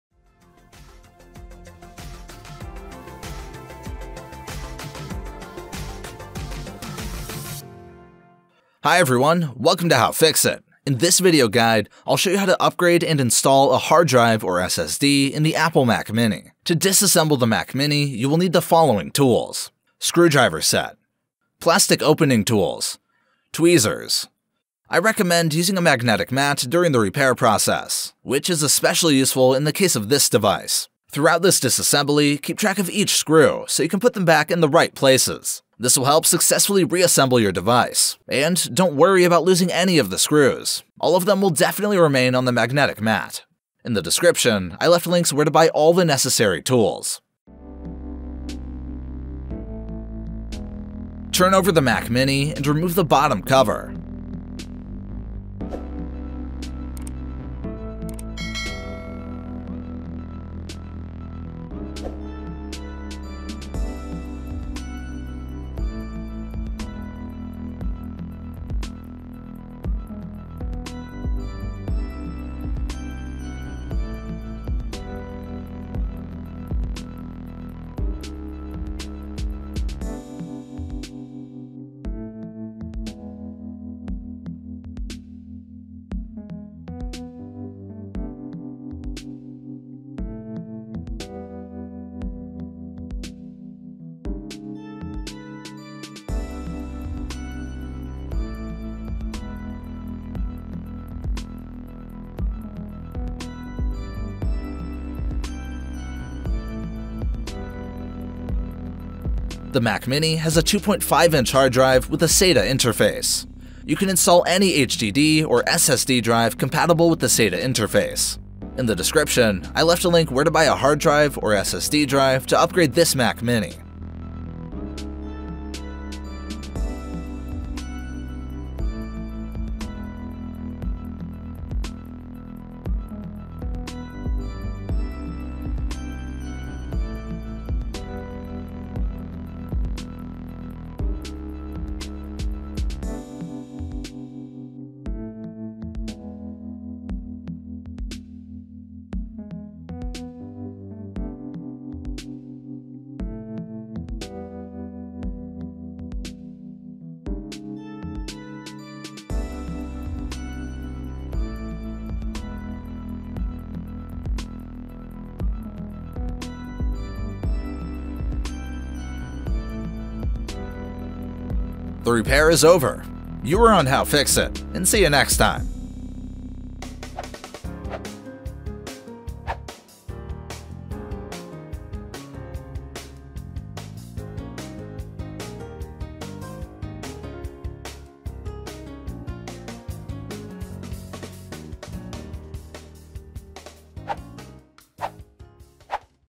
Apple Mac Mini 2011-2012 How to install SSD or Hard Drive replacement
How to upgrade SSD in laptop or replace HDD Apple Mac Mini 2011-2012